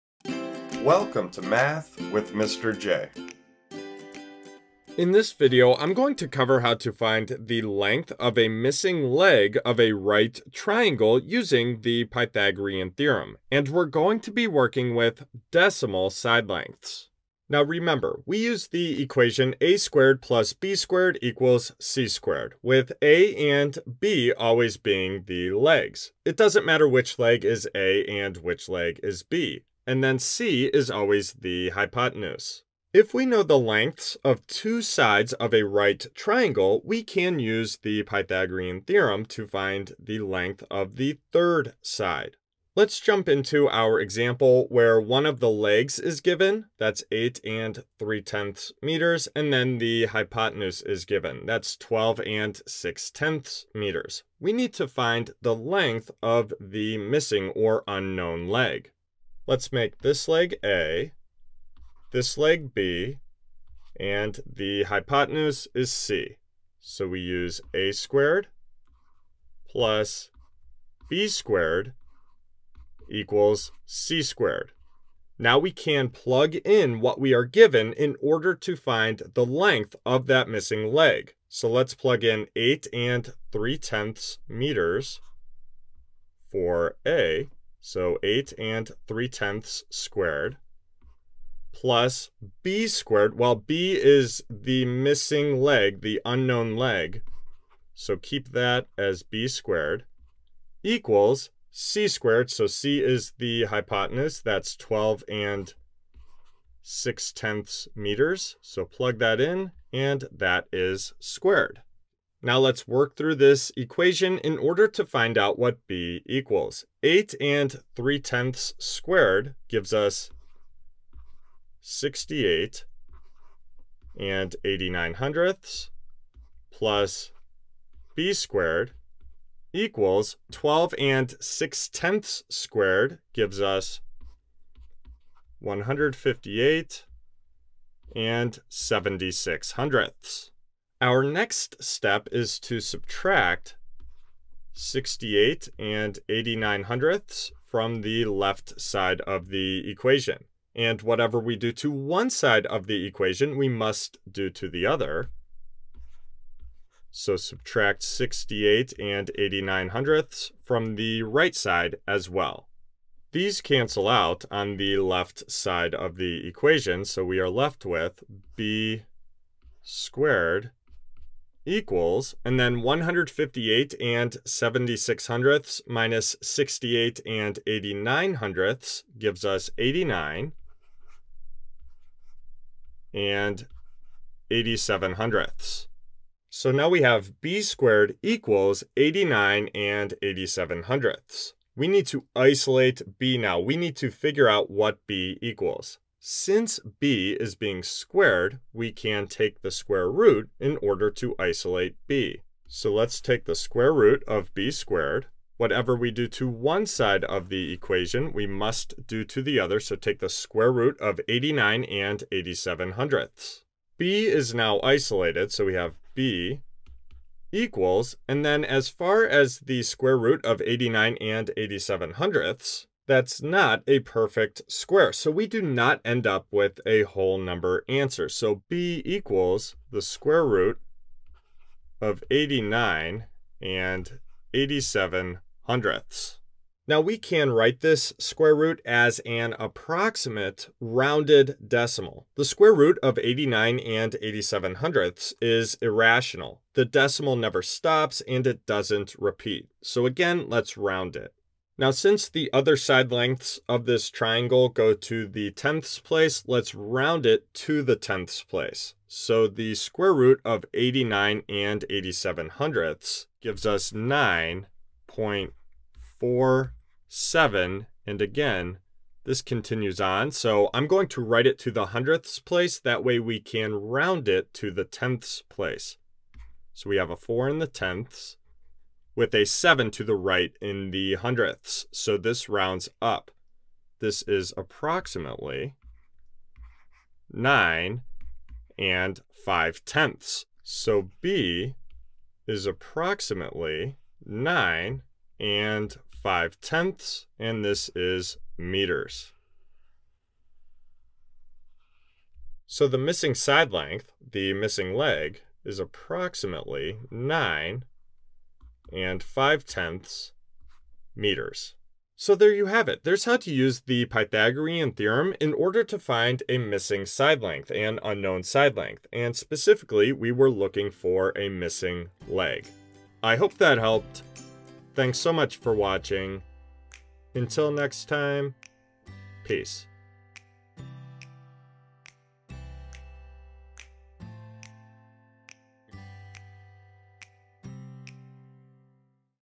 Pythagorean Theorem: Finding the Length of a Missing Leg (with Decimals) | A Step-by-Step Guide
Welcome to The Pythagorean Theorem: Finding the Length of a Missing Leg (with Decimals) with Mr. J! Need help with the Pythagorean Theorem? You're in the right place!

Whether you're just starting out, or need a quick refresher, this is the video for you if you're looking for help with the Pythagorean Theorem. Mr. J will go through two examples of right triangles and explain how to use the Pythagorean Theorem to find a missing side length.

Hopefully this video is what you're looking for when it comes to the Pythagorean Theorem.